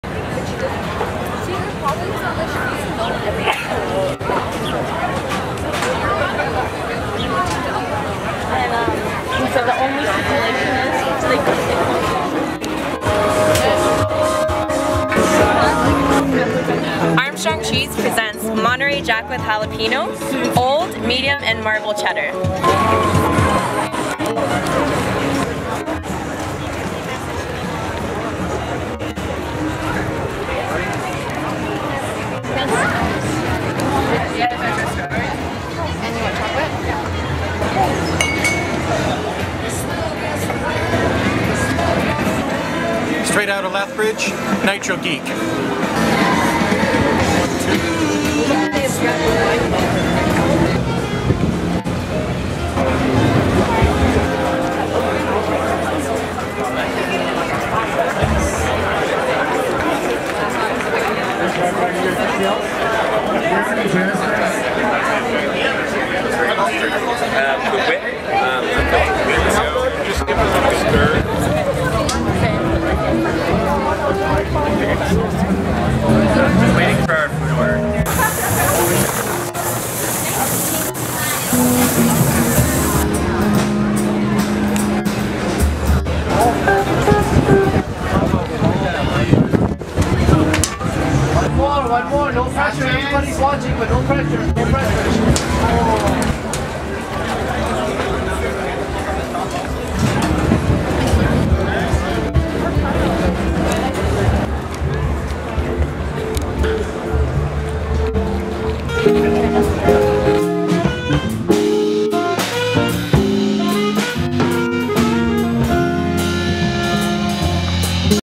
Taste Of Calgary 2015 Day2
Second day of Taste of Calgary and another hot day. Lots of different foods and drinks to try out. Vendors also selling goods.